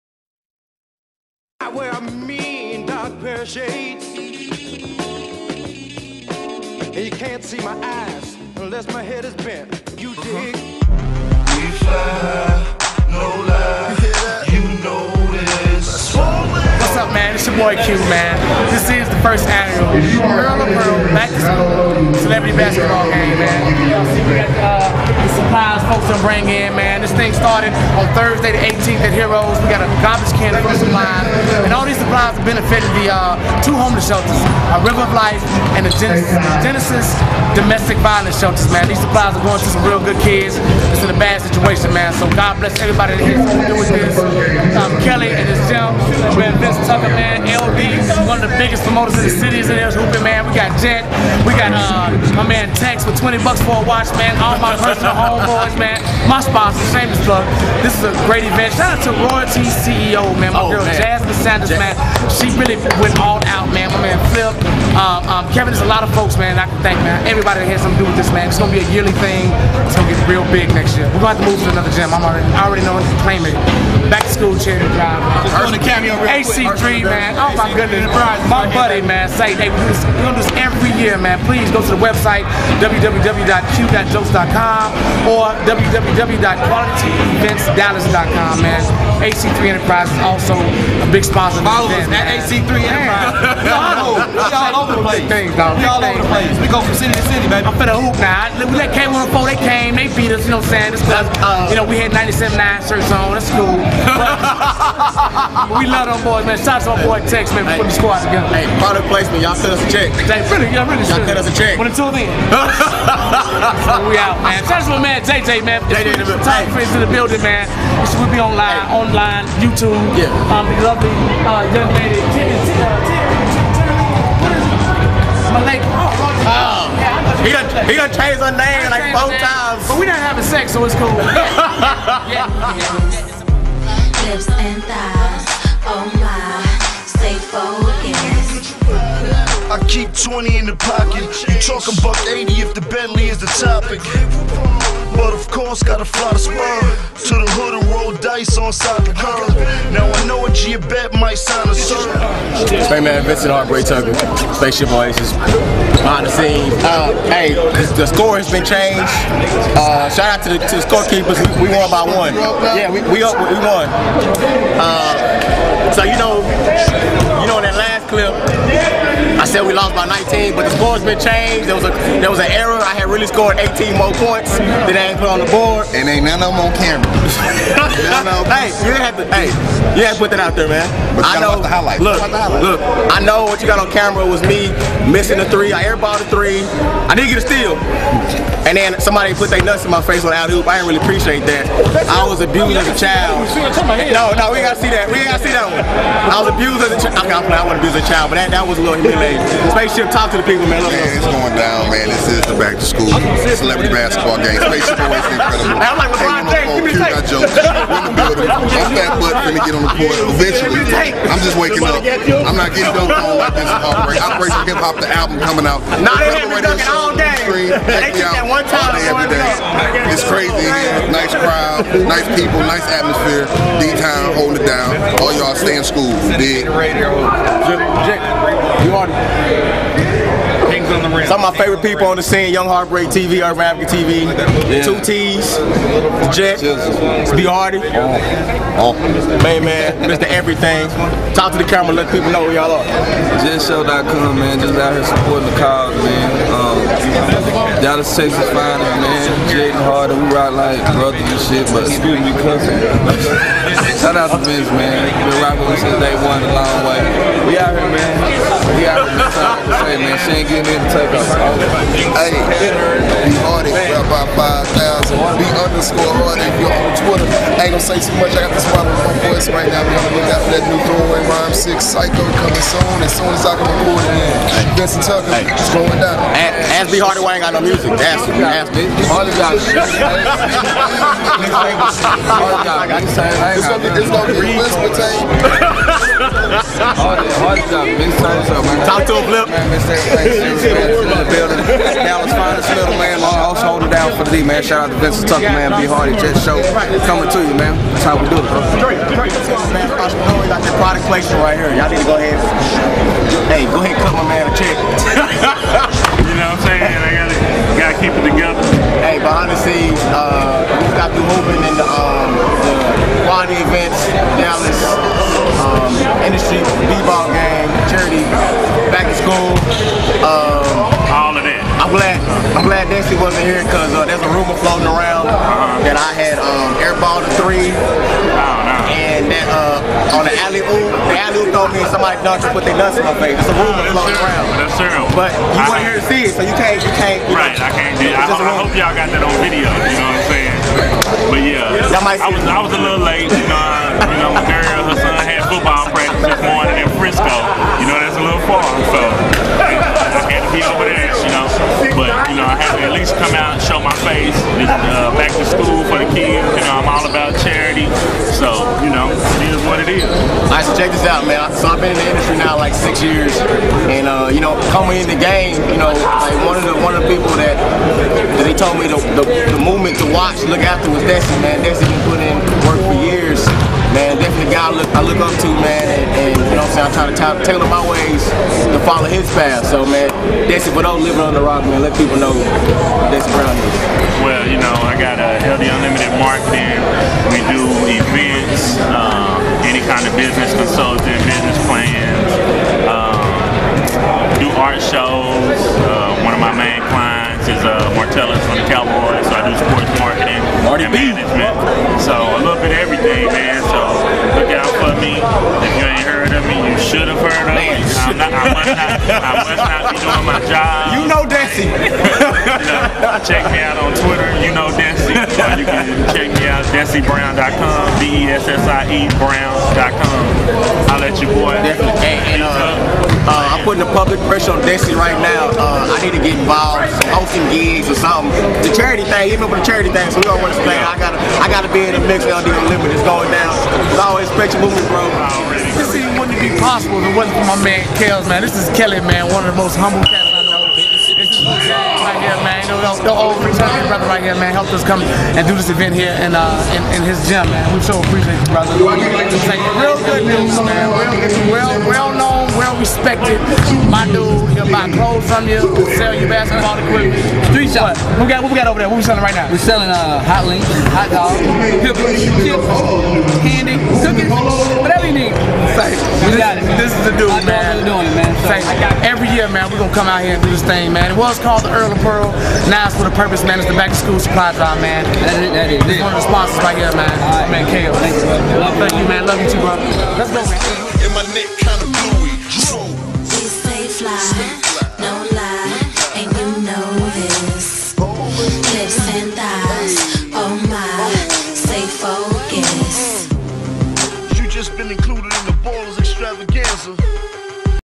The Back 2 School Industry Charity B-Ball Game & Concert Part 2
In this installment of The Underground Access, THEURBANADVOCATE.COM takes you to the Industry B-Ball Game & Concert; Team K104-vs-Team Q Got Jokes, presented by Comedian Q and Quality Events. Hosted by Vincent "Young Heartbreak" Tucker (ABC, FOX & The CW) of CWC Entertainment LLC and The Vincent Tucker Radio Show.

Videography by Remon Johnson (TheUrbanAdvocate.com)